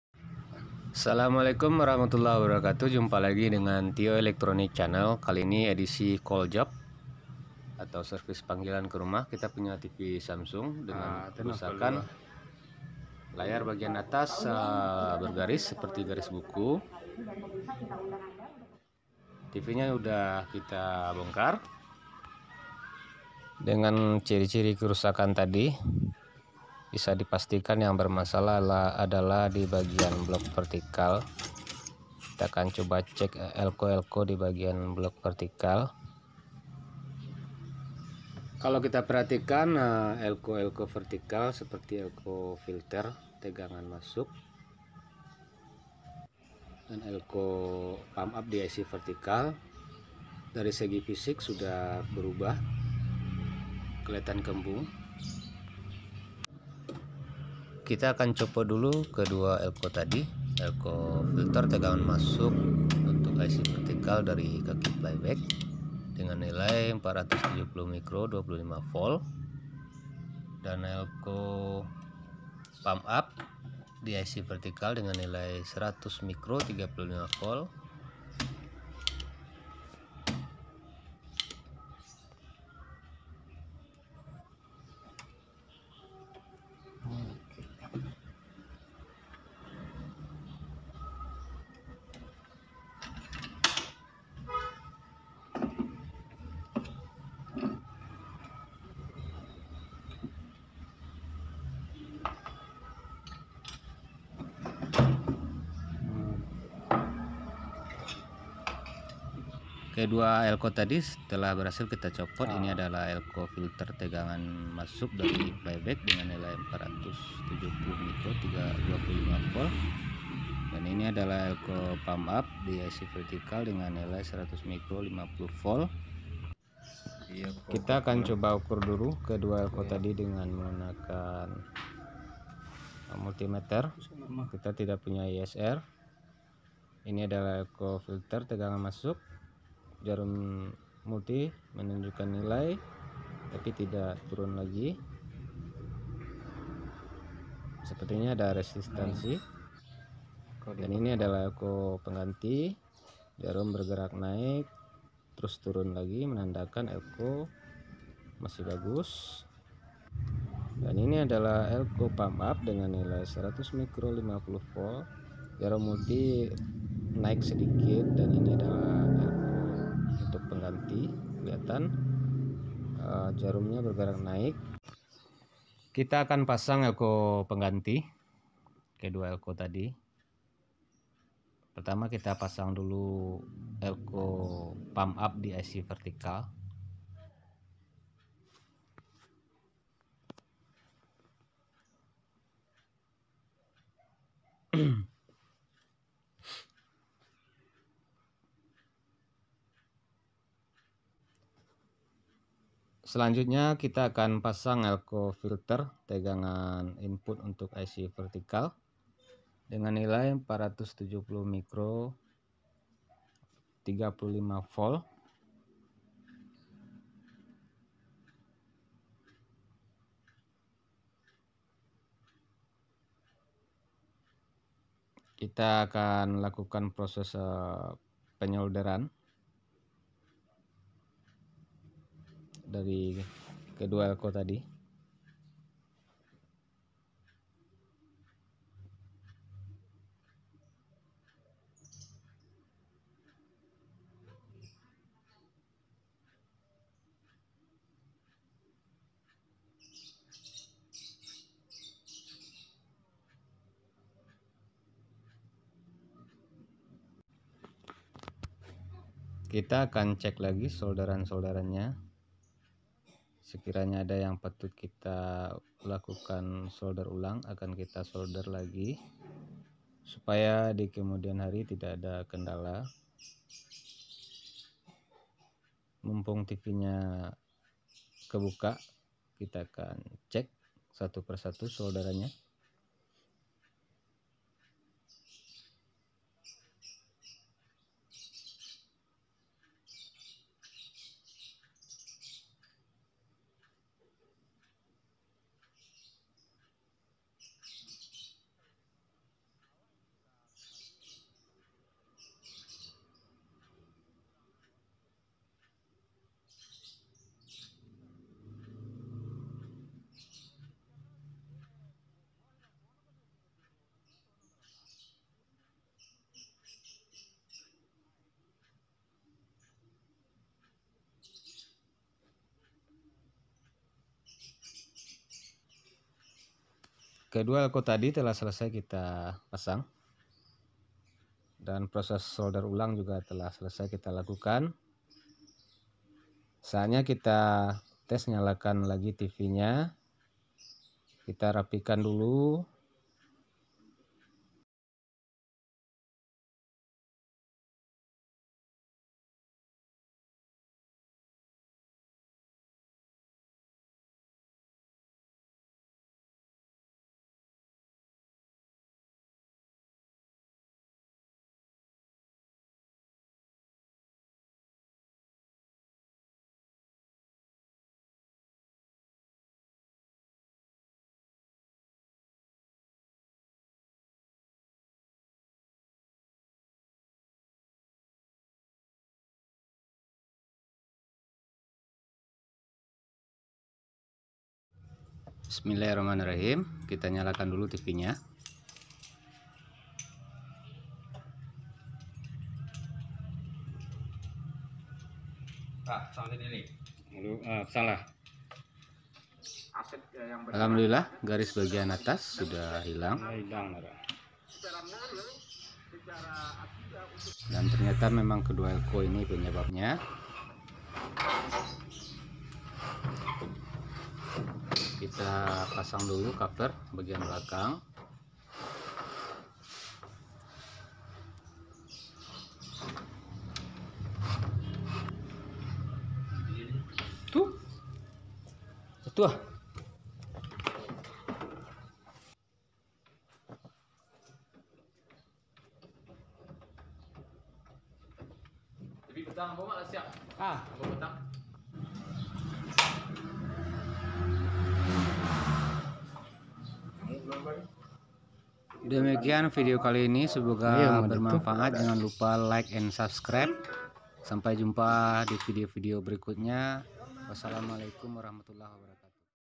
TV Samsung 29 Layar Atas Bergaris | Call Job
Kali ini Tio Electronic Channel dapat panggilan servis di tempat / Call Job.

Tv Samsung 29 " dengan kerusakan layar bagian atas bergaris/garis buku/garis tipis, dengan ciri ciri kerusakan tersebut bisa di pastikan yang bermasalah adalah di bagian Blok Vertikal.

Setelah di perhatikan ternyata Elco-elco di bagian vertikal dari segi fisik telah berubah,kembung,mengkerut.

Elco filter tegangan vertikal 470/25V dan Elco Pump Up IC vertikal 100/35 berkurang kapasitansi nya.

Dengan mengganti kedua Elco
tersebut,Garis atas layar hilang dan TV bisa normal kembali

Tio Electronic Channel, TVsamsung 29, Layar atas bergaris, Tv SamsungLayar Atas Bergaris Buku, Tv Layar bagian Atas Bergaris